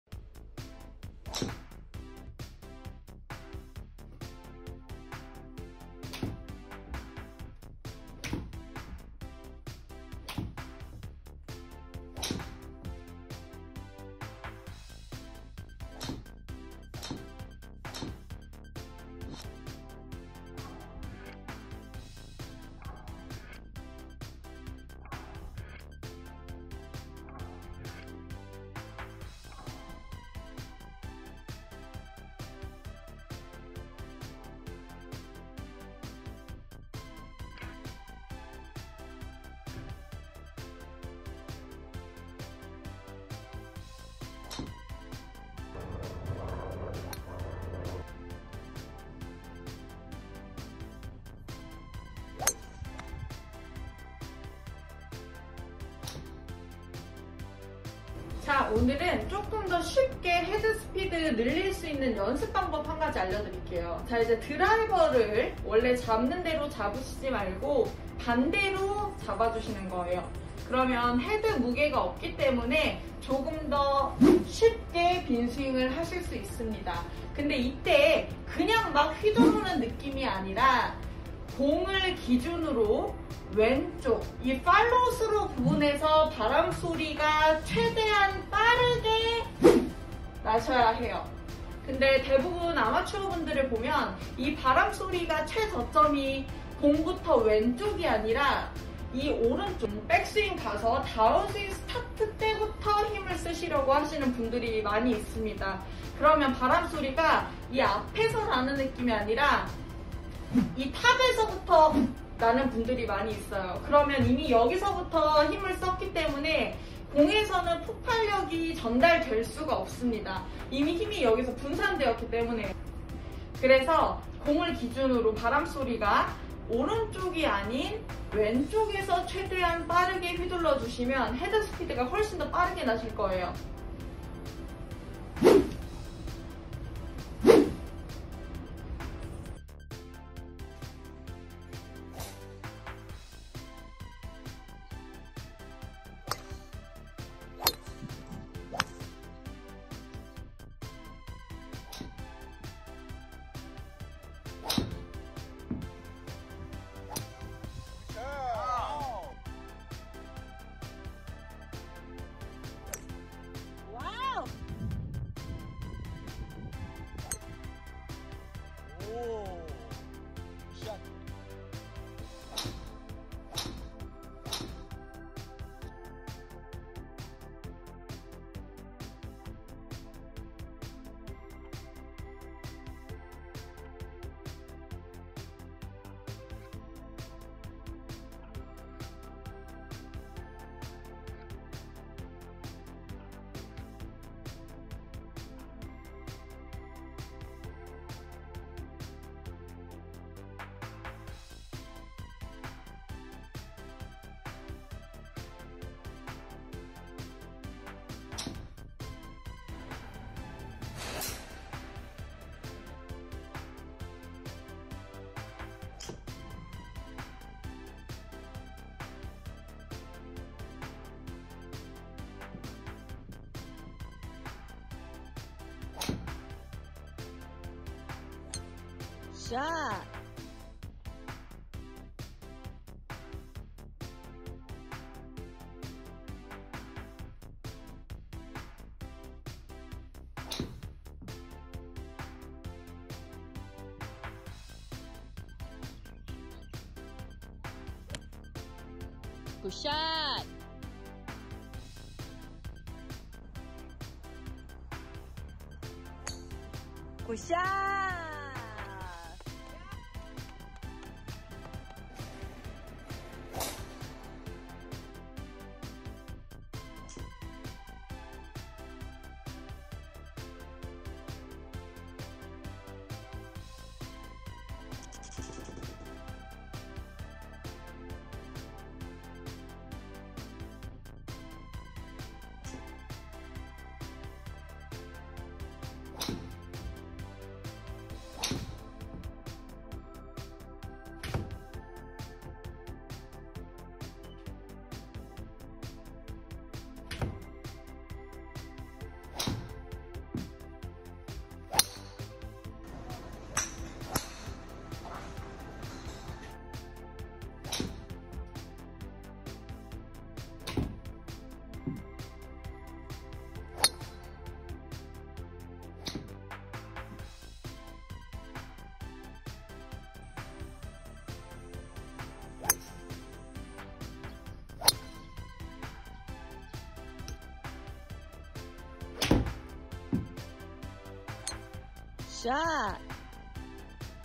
보기만 해도 헤드스피드 증가 될 것 만 같은 KLPGA 프로 김보석 미녀골퍼의 골프레슨과 골프스윙~!
김보석프로
운동선수
- Klpga Pro Golfer

- jtbc골프
- 까스텔바작
- 네이버 1인칭골프레슨
- 전자신문 '김보석의 주얼리 골프레슨' 연재중
 필드레슨 원포인트레슨 골프강연
. 문의 DM  또는 오픈카톡

때로는
감사합니다.